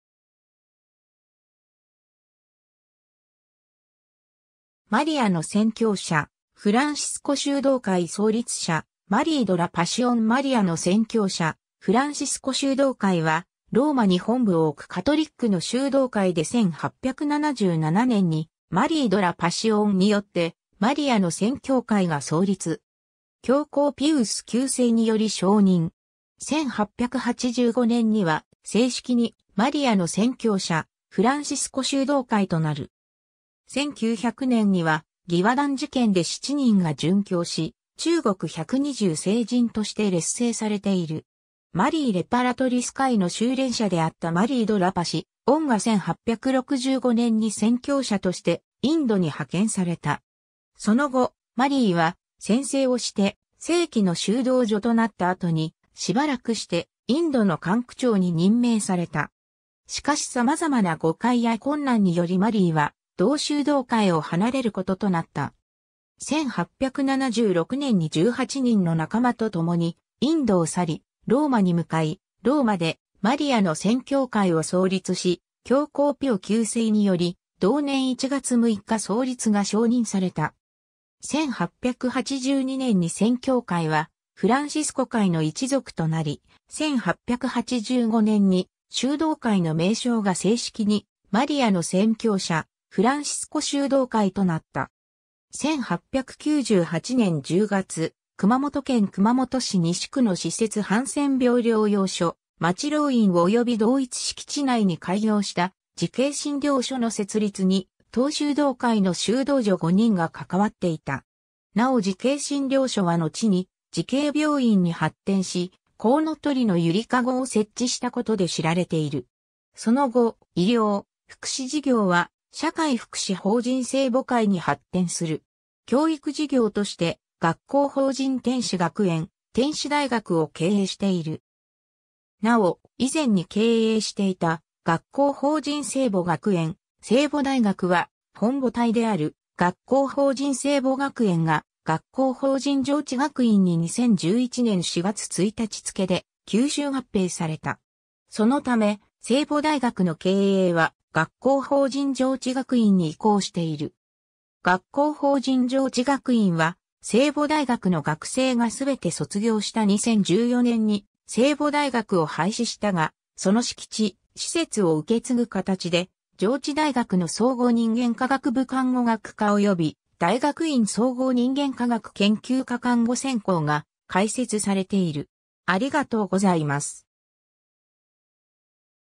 マリアの宣教者フランシスコ修道会
マリアの宣教者フランシスコ修道会創立者 マリー・ド・ラ・パシオン マリアの宣教者フランシスコ修道会（英語: Franciscan Missionaries of Mary、略称: F.M.M.）は、ローマに本部を置くカトリックの修道会で1877年にマリー・ド・ラ・パシオンによってマリアの宣教会が創立。
教皇ピウス9世により承認。
1885年には正式にマリアの宣教者フランシスコ修道会となる。
 1900年には義和団事件で7人が殉教し、中国百二十聖人として列聖されている。
 「マリー・レパラトリス会（マリアの贖罪会）」の修練者であったマリー・ド・ラ・パシオンが1865年に宣教者としてインドに派遣された。
その後、マリーは宣誓をして正規の修道女となった後に、しばらくしてインドの管区長に任命された。
しかし様々な誤解や困難によりマリーは同修道会を離れることとなった。
 1876年に18人の仲間とともにインドを去りローマに向かい、ローマで「マリアの宣教会」を創立し、教皇ピオ9世により同年1月6日創立が承認された。
1882年に宣教会はフランシスコ会の一族となり、1885年に修道会の名称が正式に「マリアの宣教者フランシスコ修道会」となった。
 1898年（明治31年）10月、熊本県熊本市西区の私設ハンセン病療養所「待労院」および同一敷地内に開業した「慈恵診療所」の設立に、当修道会の修道女5人が関わっていた。
なお慈恵診療所はのちに慈恵病院に発展し「こうのとりのゆりかご」を設置したことで知られている。
 その後、医療・福祉事業は社会福祉法人聖母会に発展する。
 教育事業として、学校法人天使学園・天使大学を経営している。
なお、以前に経営していた学校法人聖母学園・聖母大学は、本母体である学校法人聖母学園が、学校法人上智学院に2011年4月1日付で吸収合併された。
そのため、聖母大学の経営は学校法人上智学院に移行している。
学校法人上智学院は、聖母大学の学生がすべて卒業した2014年に聖母大学を廃止したが、その敷地・施設を受け継ぐ形で上智大学の総合人間科学部看護学科および大学院総合人間科学研究科看護専攻が開設されている。